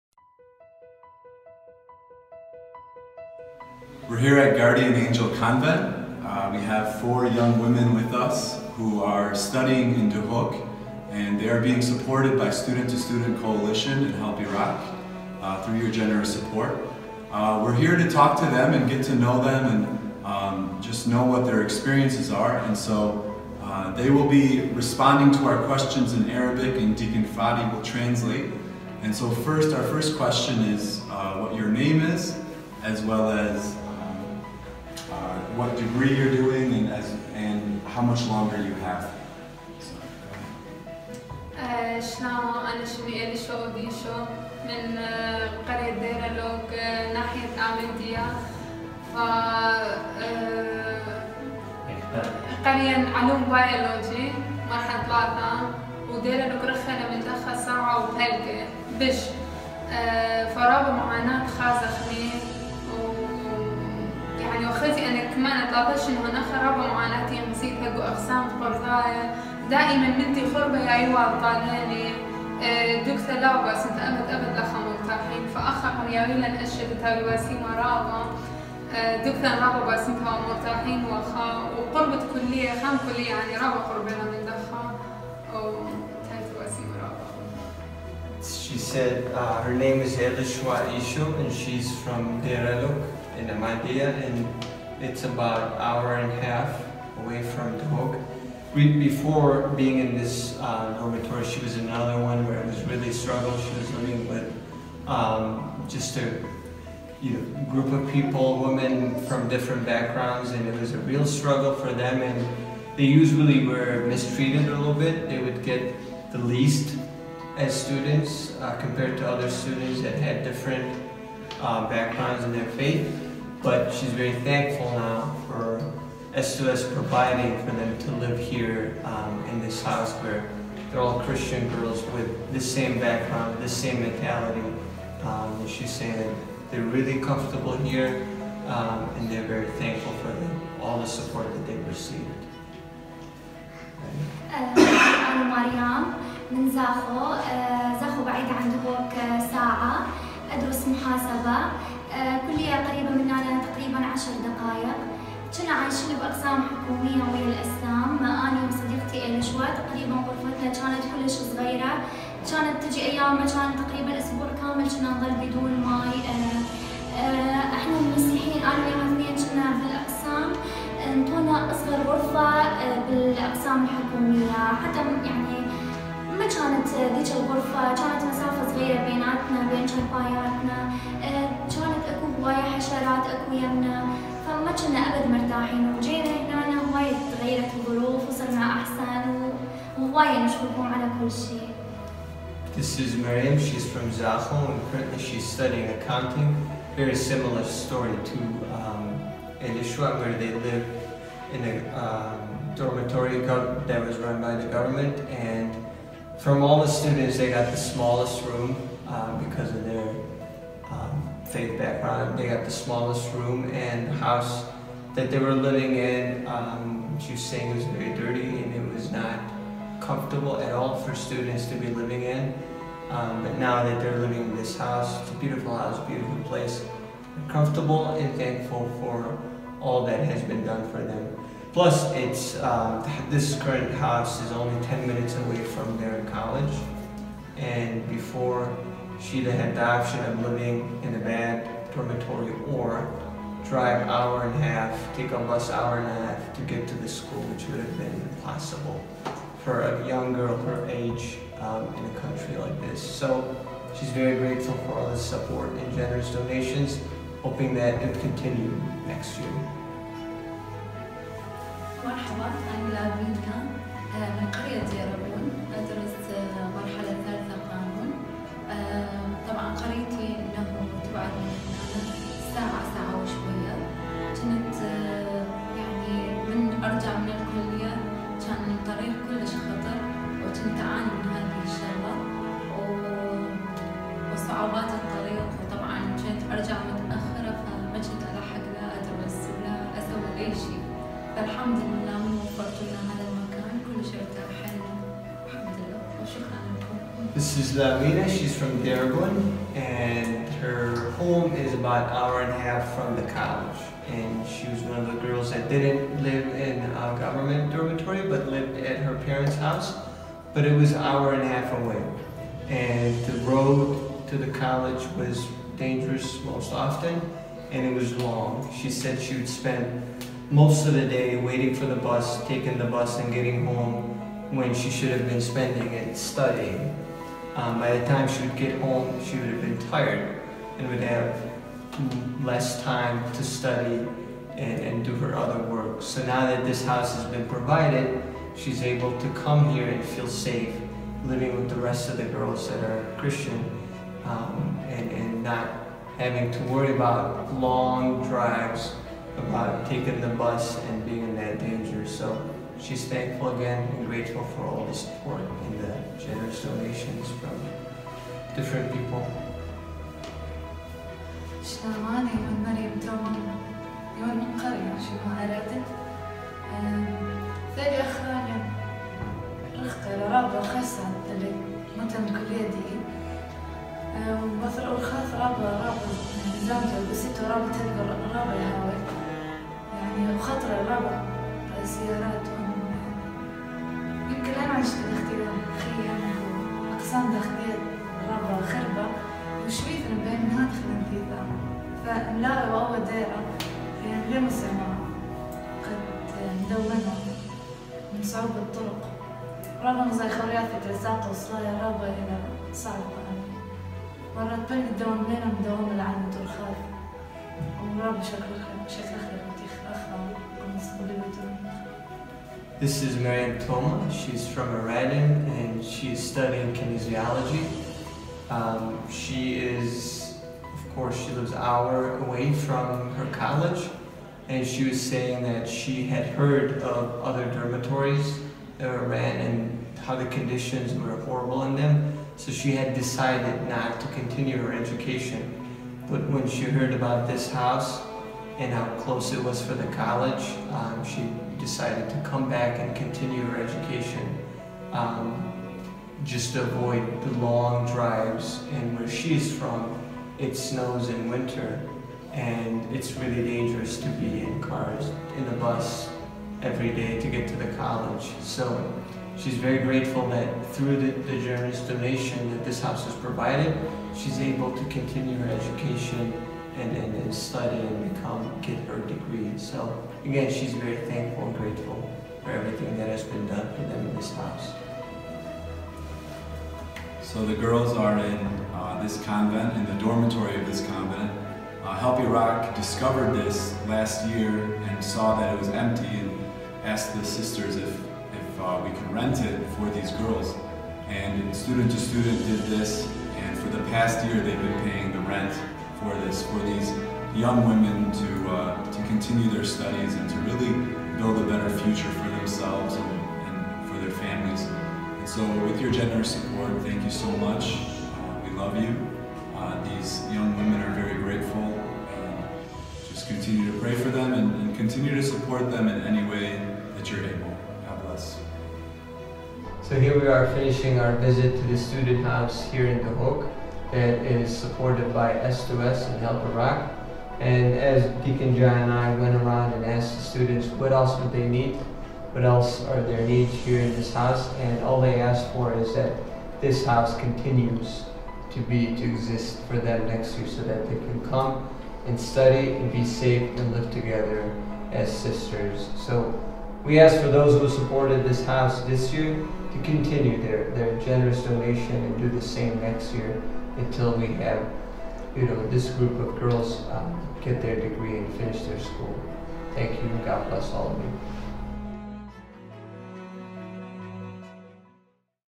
S2S Spring 2018- Dehuk Girls Interviews
Fr. John Jaddou and Fr. Fadie Georgis interviewed students whom Student2Student supports in Dehuk.